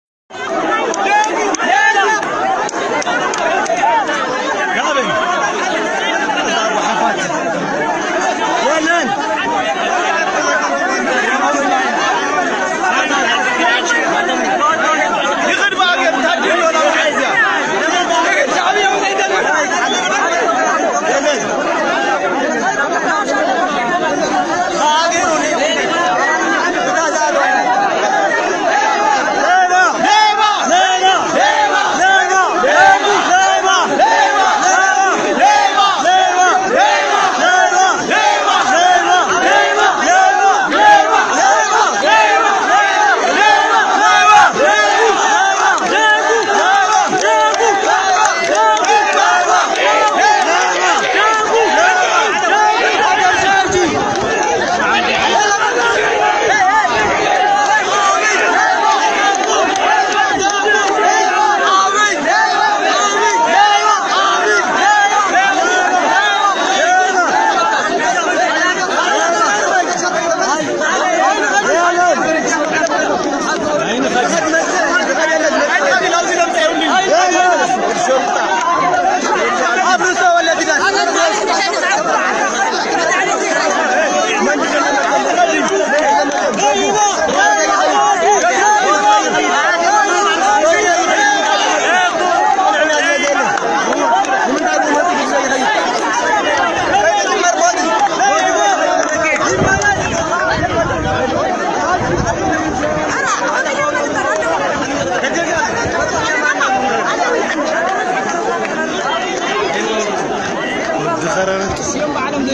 በሽሬ ከተማ ሊያልፉ የነበሩ የመከላከያ መኪናዎች ዛሬም ለሁለተኛ ግዜ በአካባቢው ወጣቶች ታግተዋል።
ወጣቶች መንገዶችን በመኪናቸው የዘጉ ሲሆን የተወሰኑት ደግሞ በአቅራቢያው ወዳለ ስታዲየም እንደተወሰዱ ምንጮች ከአካባቢው አሳውቀውናል።